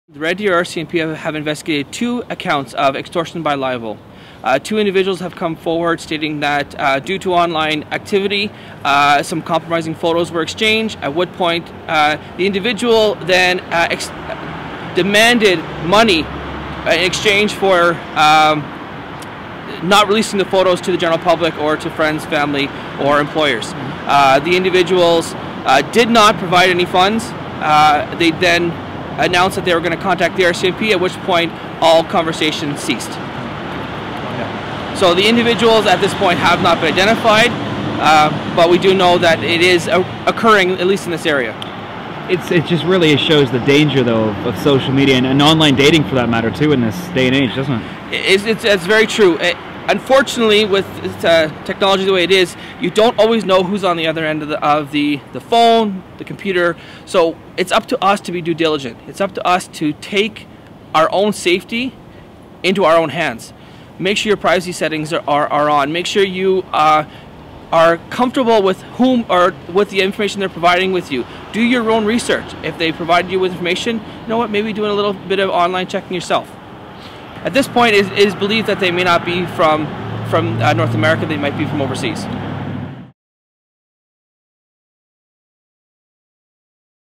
Red Deer RCMP investigate nude photo extortion case
On at least two reported occasions, men speaking with women via social media, sent lewd photographs of themselves at the request of the person on the other side of the screen, only to have the person turn around and ask for money ... or else.